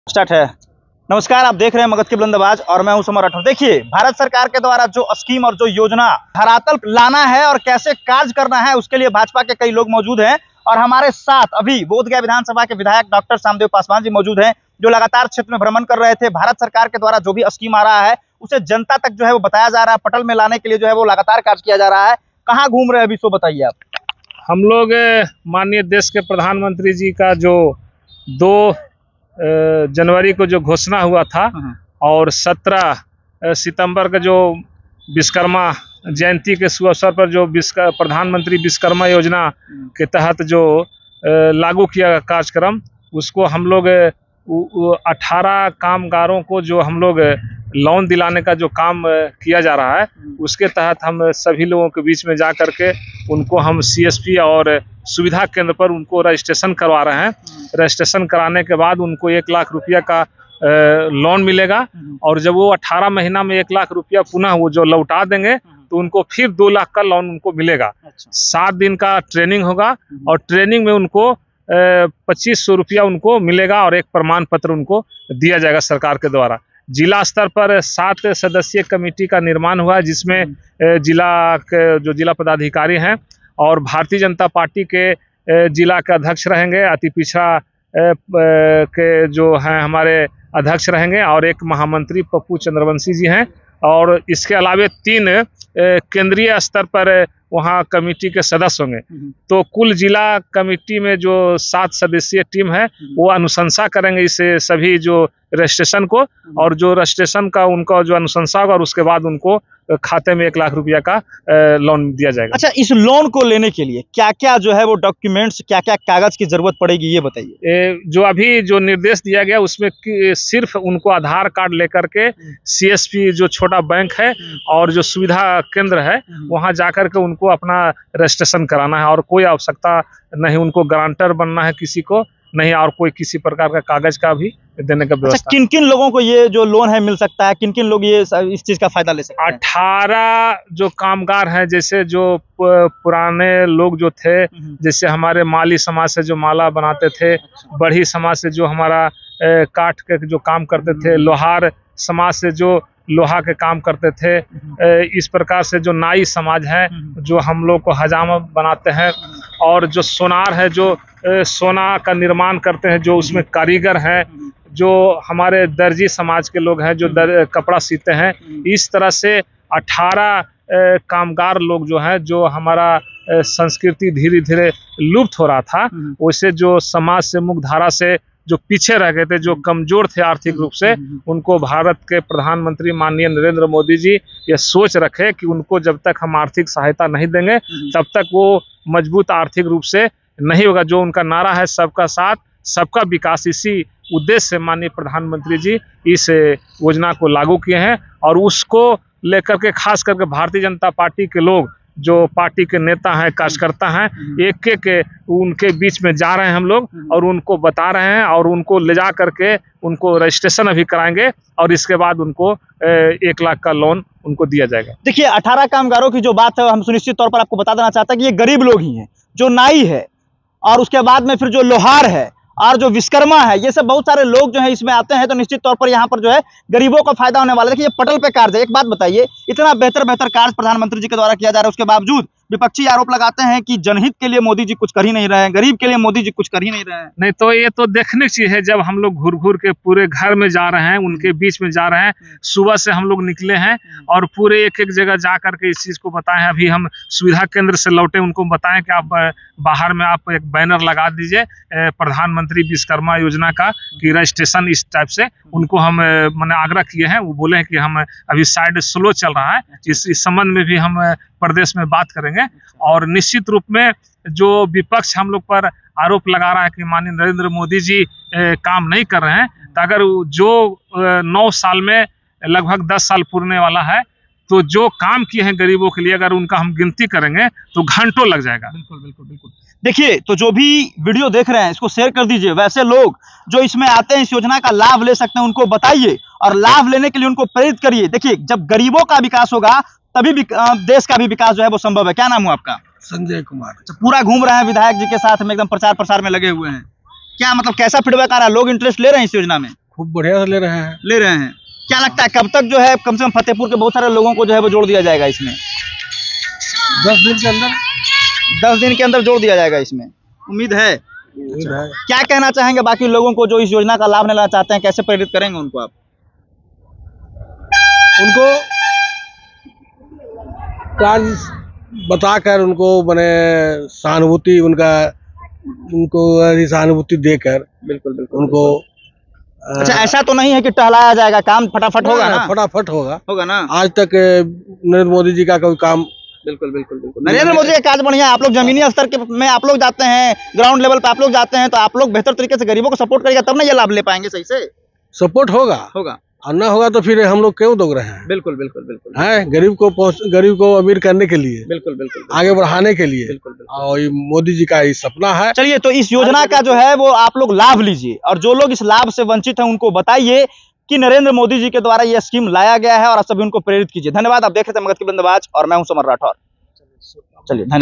करोड़ों कामगारों को विश्वकर्मा योजना के तहत जल्द मिलेंगे लाभ,बीजेपी नेता कर रहें है प्रचार, प्रसार।
बीजेपी के नेता गाँव गाँव में घूमकर कर रहे प्राचार।